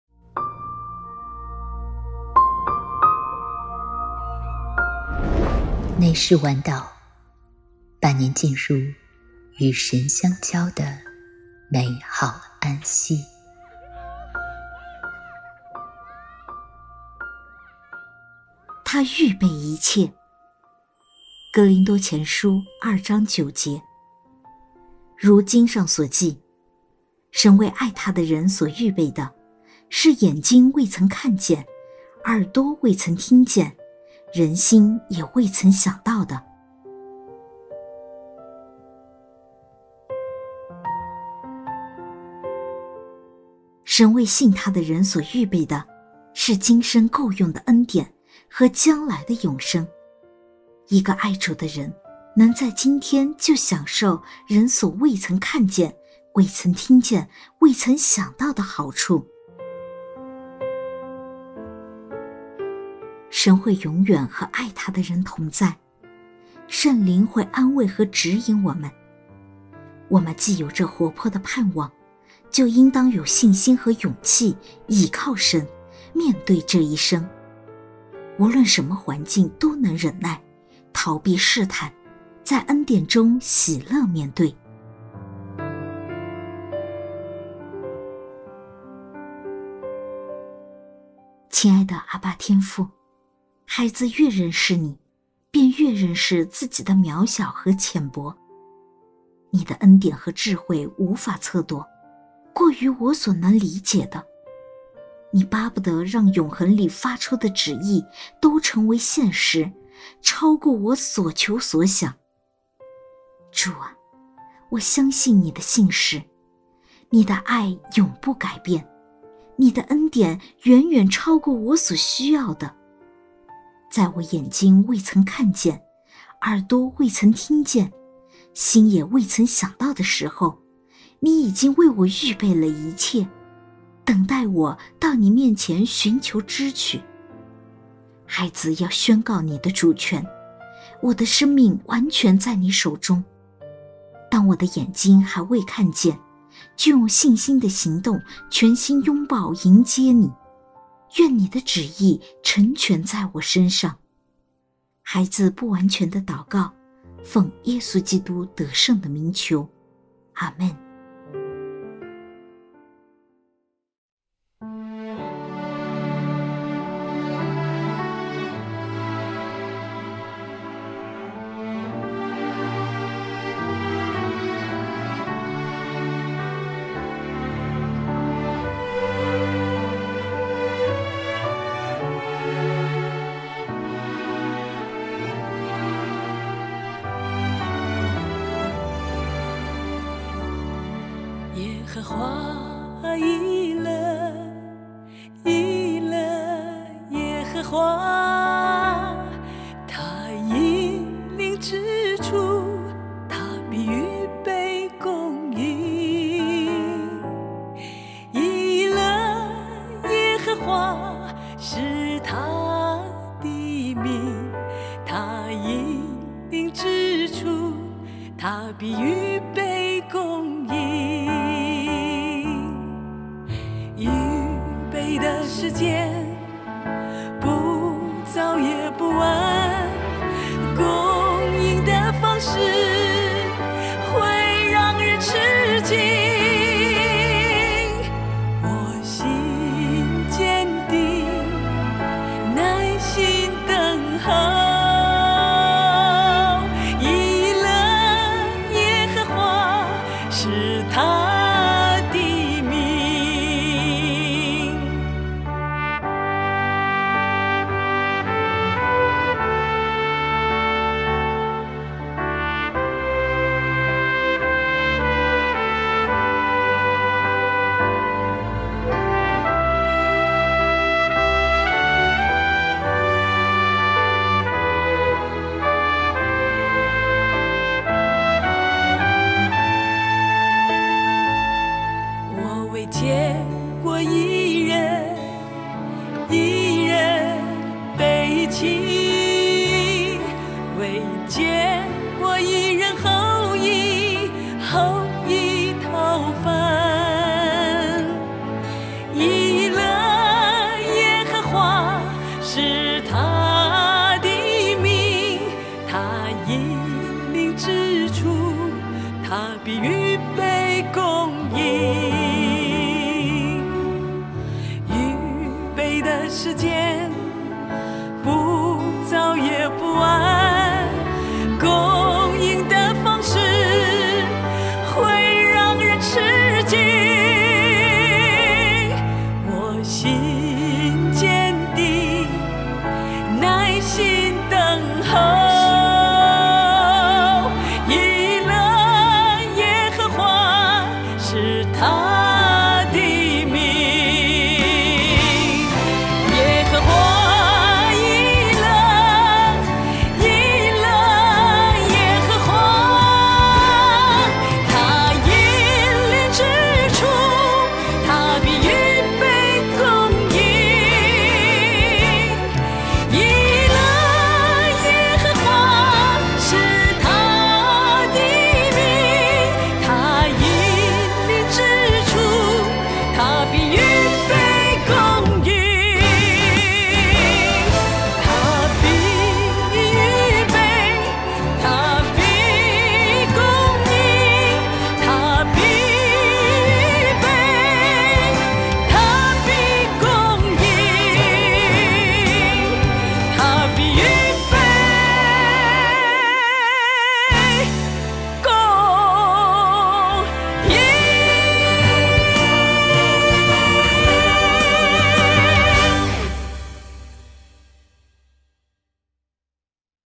祂预备一切 [2024-4-26 内室晚祷]
经文
哥林多前书2：9
如经上所记：“神为爱他的人所预备的，是眼睛未曾看见，耳朵未曾听见，人心也未曾想到的。”

观察
神为信祂的人所预备的是今生够用的恩典和将来的永生，一个爱主的人，能在今天就享受人所未曾看见、未曾听见、未曾想到的好处。

应用
神会永远与爱祂的人同在，圣灵会安慰和指引我们。我们既有这活泼的盼望，就应当有信心和勇气，倚靠神，面对这一生，无论什么环境都能忍耐，逃避试探，在恩典中喜乐面对。

祷告
亲爱的阿爸天父，孩子越认识祢，便越认识自己的渺小和浅薄。祢的恩典和智慧无法测度，过于我所能理解的。祢巴不得让永恒里发出的旨意都成为现实，超过我所求所想。

主啊，我相信祢的信实，祢的爱不改变，祢的恩典远远超过我所需要的。在我眼睛未曾看见，耳朵未曾听见，心也未曾想到的时候，祢已经为了我预备了一切，等待我到祢面前寻求支取。

孩子要宣告祢的主权，我的生命完全在祢手中，当我的眼睛还未看见，就用信心的行动全心拥抱迎接祢，愿祢的旨意成全在我身上。孩子不完全的祷告，奉耶稣基督得胜的名求，阿们！

内室晚祷#737｜（文：丽霞  录音：子规 后期：Daniel  选诗：娜娜）